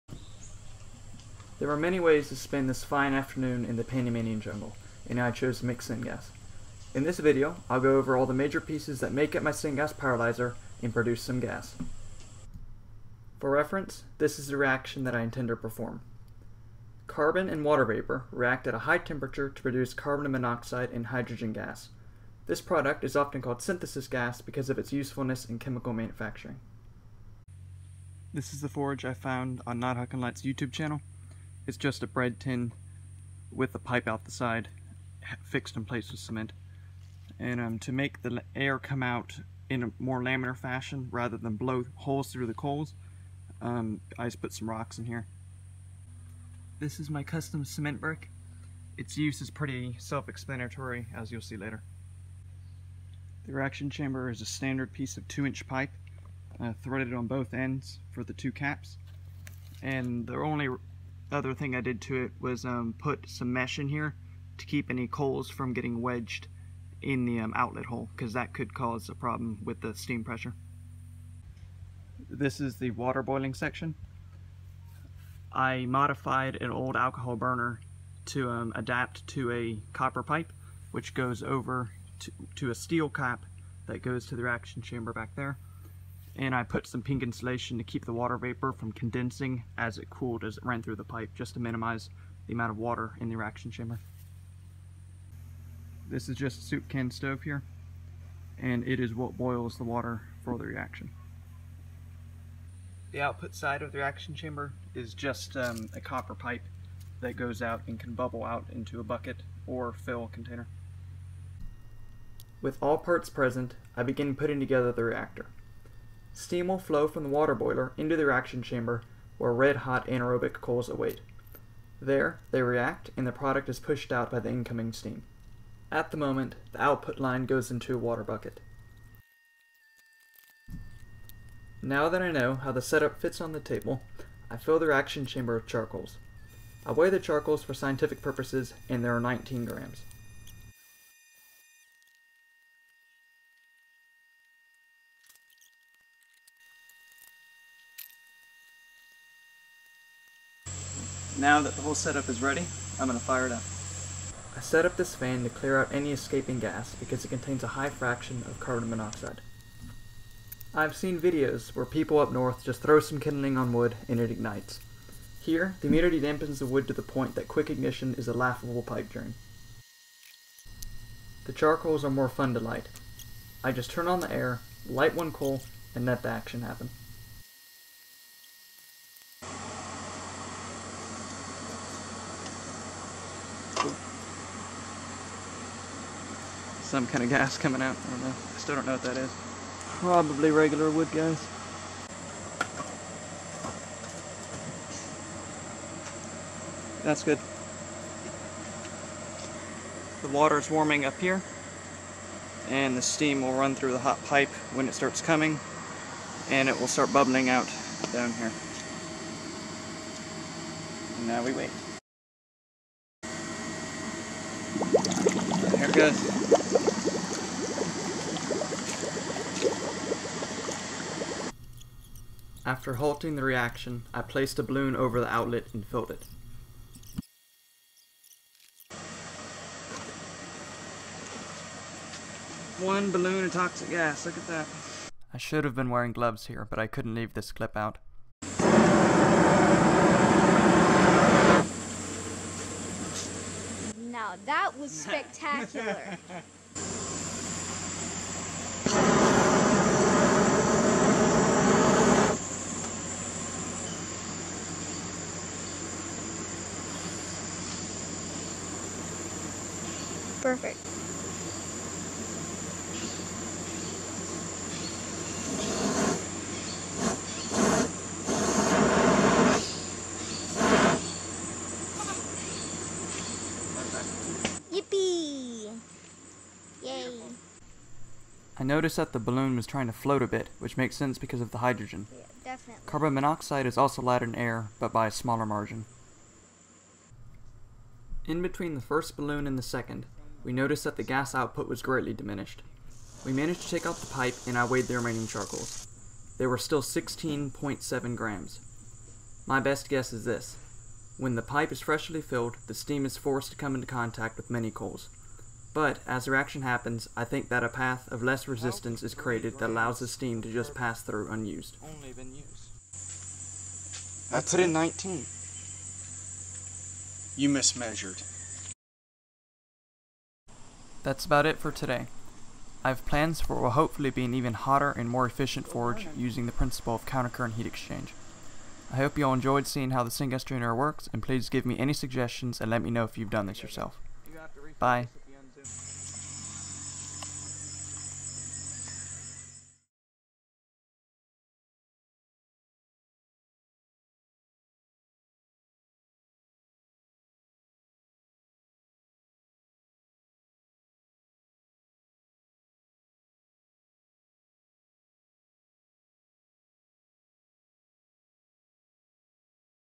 Homemade Syngas Generator
In this video, I lay out the construction and function of my syngas generator. I also burn some gas!

Syngas is a mixture of carbon monoxide and hydrogen. It is a useful feed-stock in chemical manufacturing. I have considered making diesel fuel out of it, but I think that pressurized vessels are currently beyond my capability.

An interesting note is that the syngas produced here is a bio-fuel. I am simply taking water and carbon from wood and converting into gas. Even when burned, this is technically carbon-neutral because if *I* hadn't converted it to carbon dioxide, fungi and termites would have! (with the exception of the ignition sources.) While this is technically clean, I am fully aware that this is extremely inefficient and not practical. Just a tidbit!

Sometime soon I'll melt some table salt with the pyrolizer.

If you would like to see where I live, look up "Tranquilo Bay." It is in Panama, the country. The monkeys happened to be there when I was setting up.